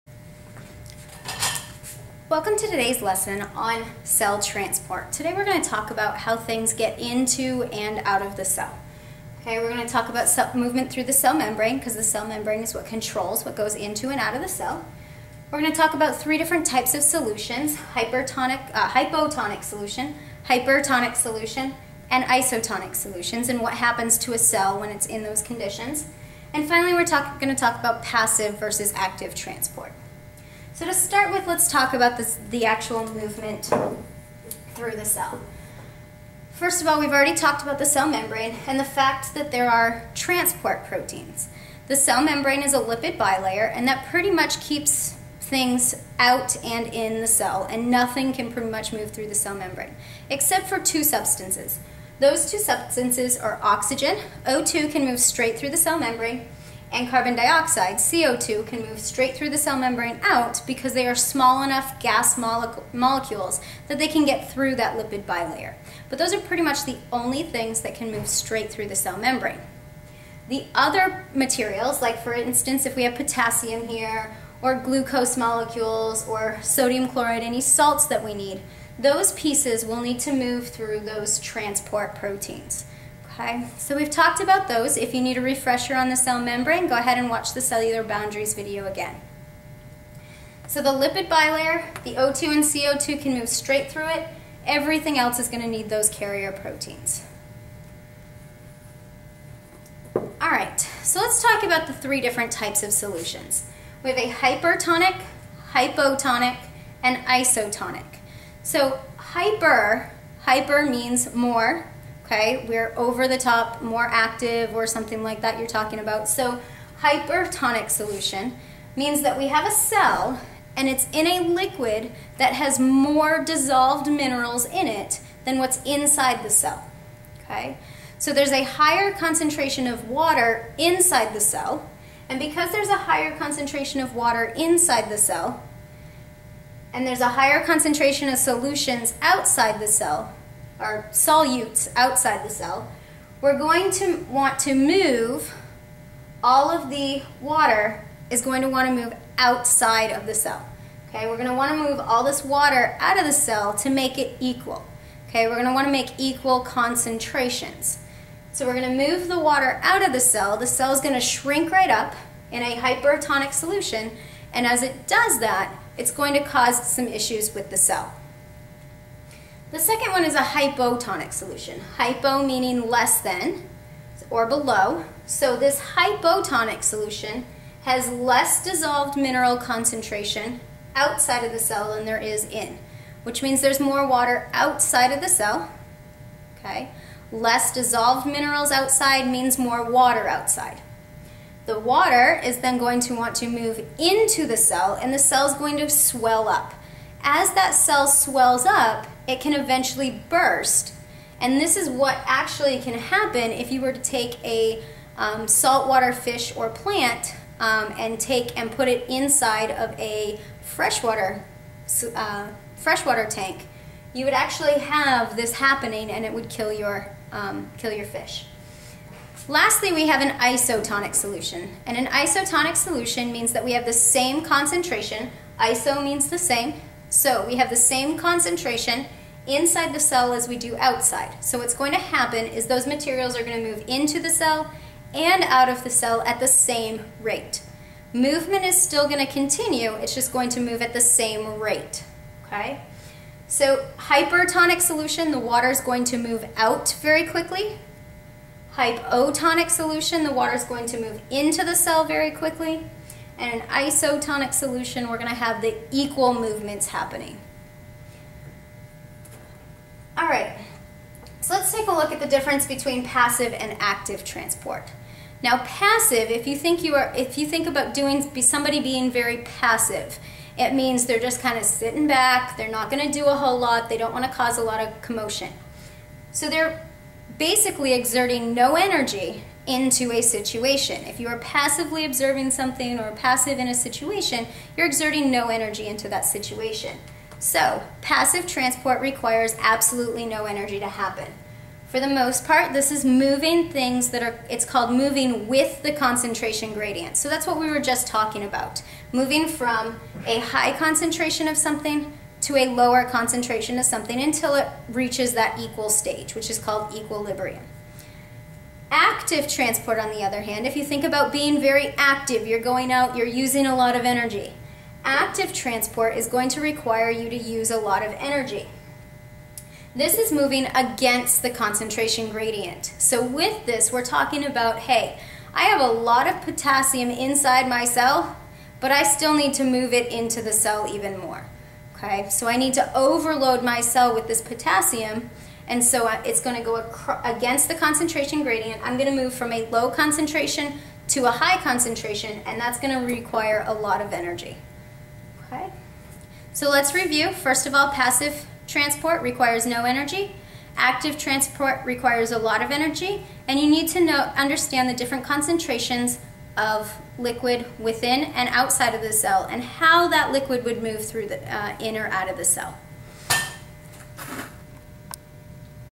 Cell Transport
This video lecture discusses how materials are moved into and out of the cell.  It also discusses the vocabulary of isotonic, hypotonic, and hypertonic solutions and what happens to a cell when they are placed in these solutions.